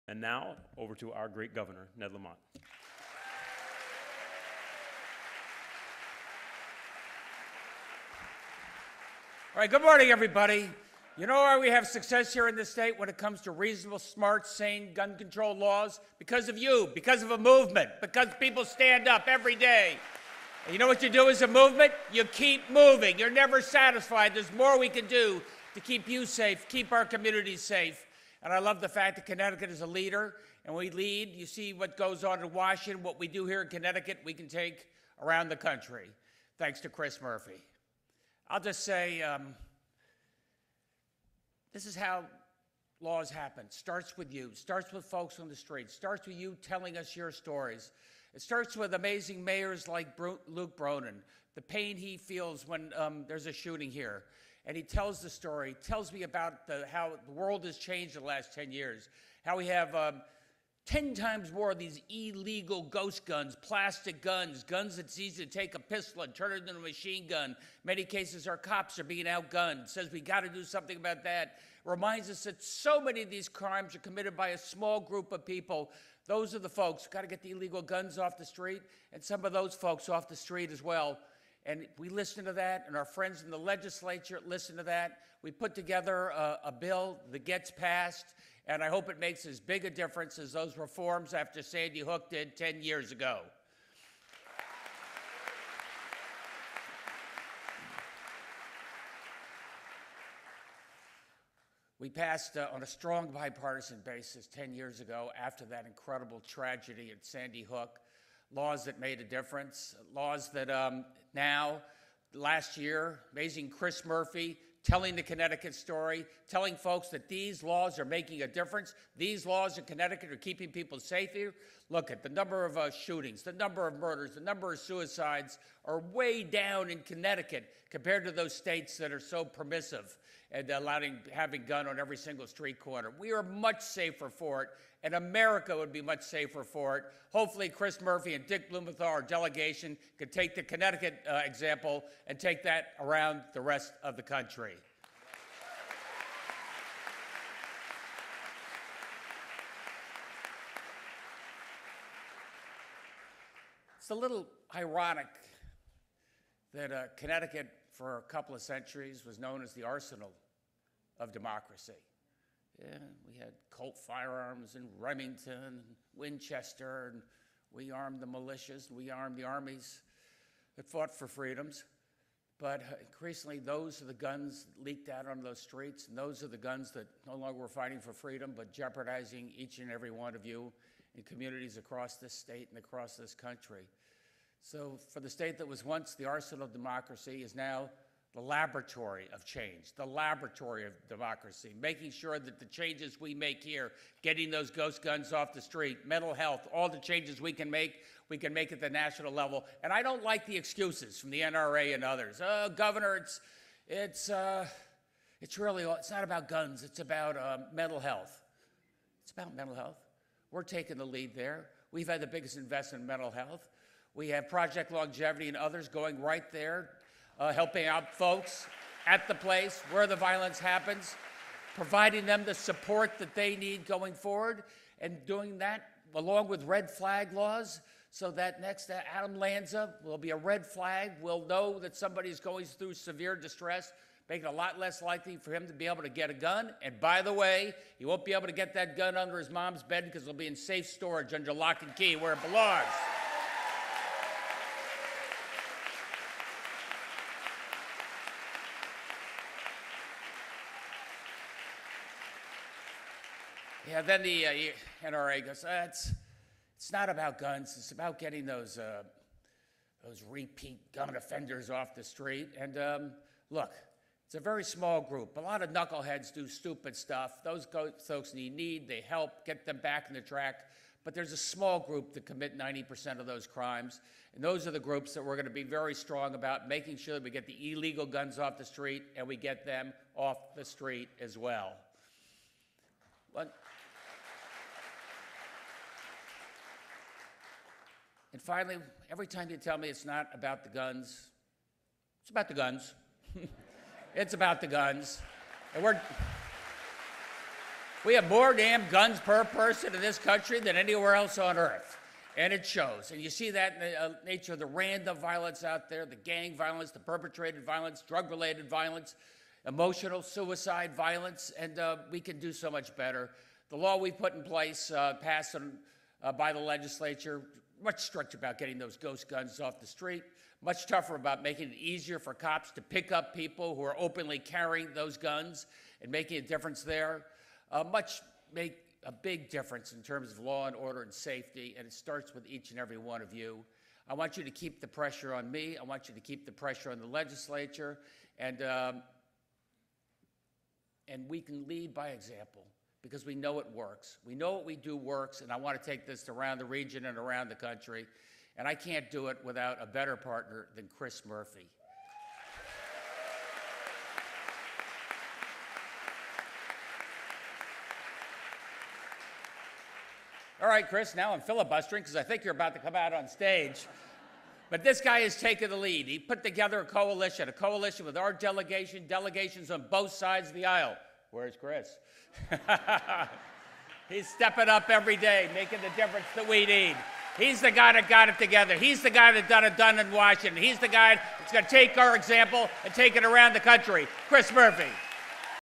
Gov. Ned Lamont addresses crowd at National Safer Communities Summit
Connecticut Gov. Ned Lamont welcomes guests to the National Safer Communities Summit, hosted at Univ. of Hartford.